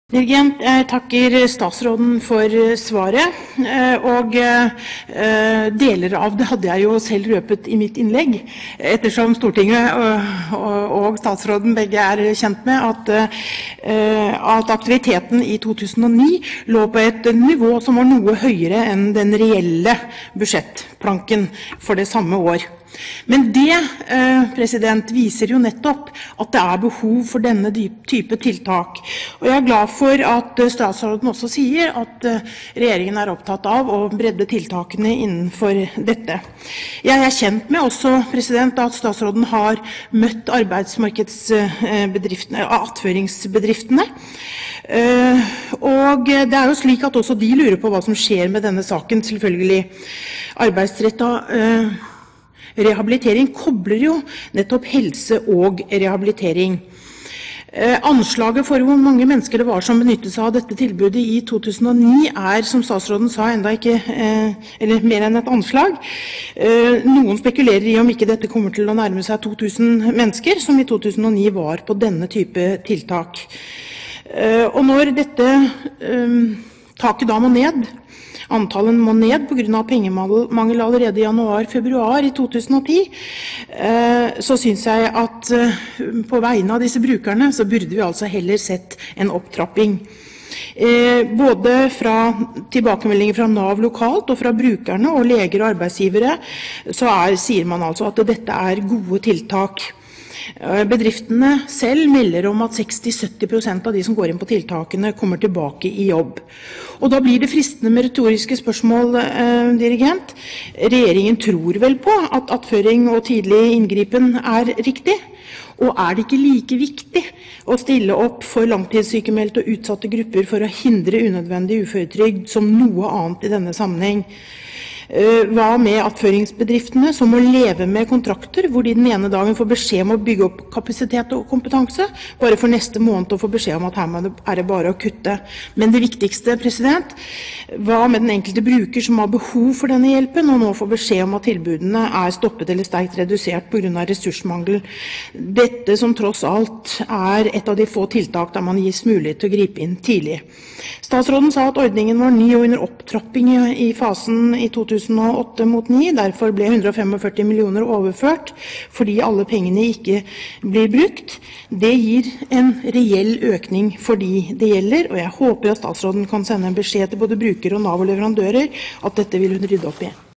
02.03.2010 Sak 3 2 Sylvi Graham 2
Om kutt i tilbudet om arbeidsrettet rehabilitering, og hva man vil gjøre for å gjenopprette troverdigheten og sørge for at syke mennesker kan få en ny sjanse til arbeid